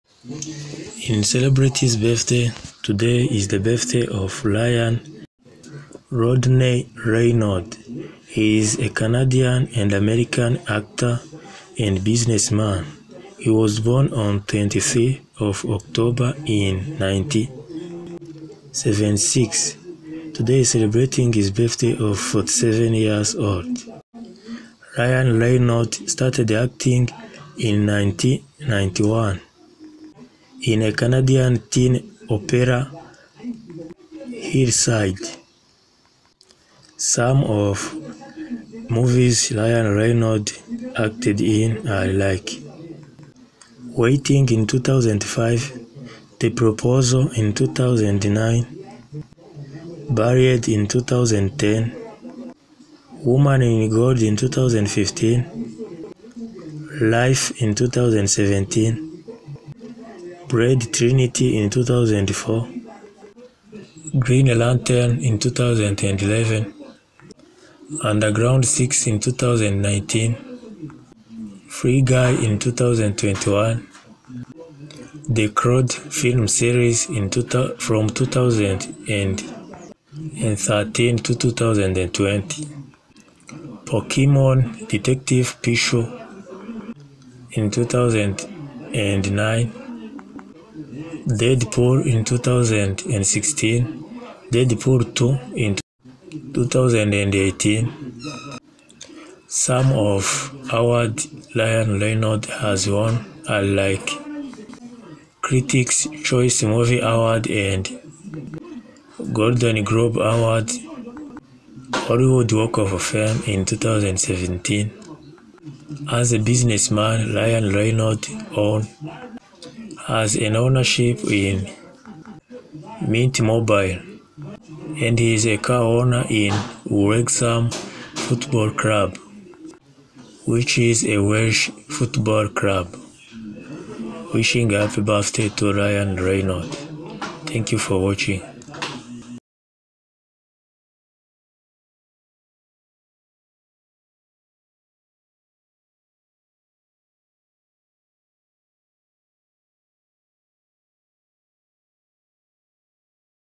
Ryan Reynolds is 47 years old /happybirthday / Bonne anniversaire à ryanreynolds /isabukuru nziza
RYAN Rodney Reynolds uzwinka ryanreynolds ni umunya America wumu kinnyi wama film, yavutse tarikiya 23/10/1976.
uyumunsi yajyize isabukuru yamavuko yimyaka 47

Ryan Rodney Reynolds OBC (born October 23, 1976) is a Canadian and American actor, producer and businessman. He began his career starring in the Canadian teen soap opera Hillside (1991–1993), and had minor roles before landing the lead role on the sitcom Two Guys and a Girl between 1998 and 2001. Reynolds then starred in a range of films, including comedies such as National Lampoon's Van Wilder (2002), Waiting... (2005), and The Proposal (2009). He also performed in dramatic roles in Buried (2010), Woman in Gold (2015), and Life (2017), starred in action films such as Blade: Trinity (2004), Green Lantern (2011), 6 Underground (2019), and Free Guy (2021), and provided voice acting in the animated features The Croods film series (2013–2020), Turbo (2013), and Pokémon: Detective Pikachu (2019).
Reynolds's biggest commercial success came with the 20th Century Fox X-Men films Deadpool (2016) and Deadpool 2 (2018), in which he played the title character. The former set numerous records at the time of its release for an R-rated comedy and his performance earned him nominations at the Critics' Choice Movie Awards and the Golden Globe Awards.

Reynolds was named People's Sexiest Man Alive in 2010 and was awarded a star on the Hollywood Walk of Fame in 2017. As a businessman, he holds an ownership stake in Mint Mobile and is a co-owner of Welsh football club Wrexham A.F.C.; the latter was documented in the TV series Welcome to Wrexham. In 2020, Reynolds sold his ownership stake of Aviation Gin to Diageo for $610 million.[1] He also sits on the board of the Match Group.[2] As of 2023, his net worth is estimated to be $350 million.[3]